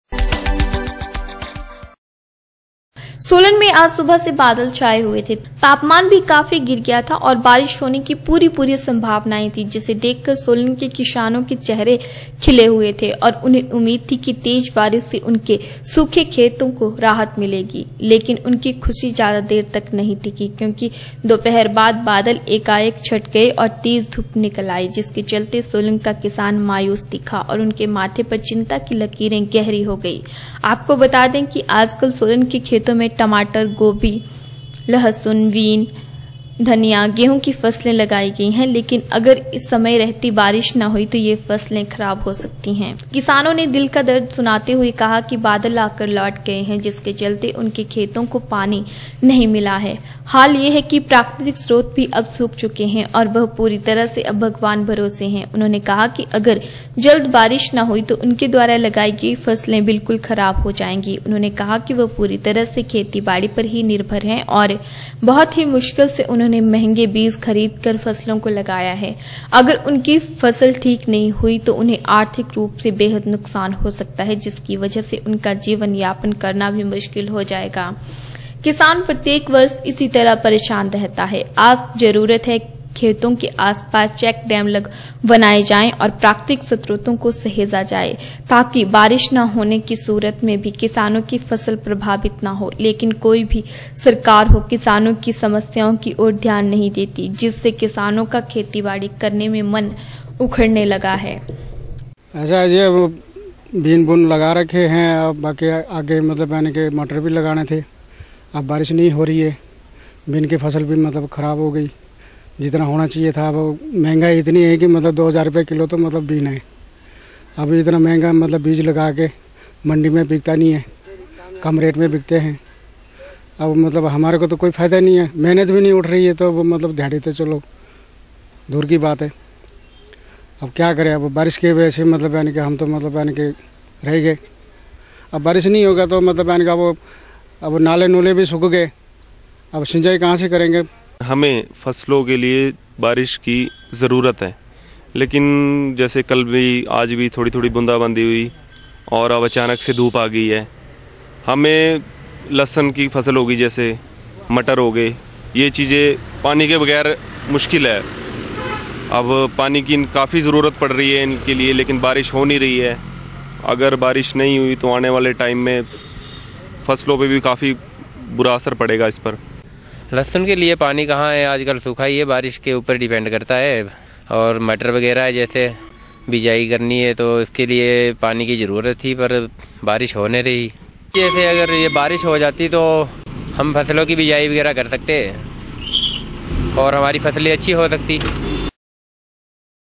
वारिश ना होने से सोलन का किसान दिखा मायूस , माथे पर चिंता की लकीरें गहरी
सोलन में आज सुबह से बादल छाए हुए थे  तापमान भी  काफी गिर गया था और बारिश  होने की पूरी पूरी सम्भावनाएं थी जिसे देख कर सोलन के किसानों के चेहरे भी खिले हुए थे और उन्हें उम्मीद थी कि तेज़ बारिश से उनके सूखते खेतों  थोड़ी राहत मिलेगी  लेकिन उनकी ख़ुशी ज्यादा देर तक नहीं टिकी क्योंकि कि दोपहर बाद बादल एकाएक छंट गए और तेज़ धुप निकल आई  जिसके चलते सोलन का किसान मायूस दिखा और उनके माथे पर  चिंता की लकीरें  गहरी हो गई |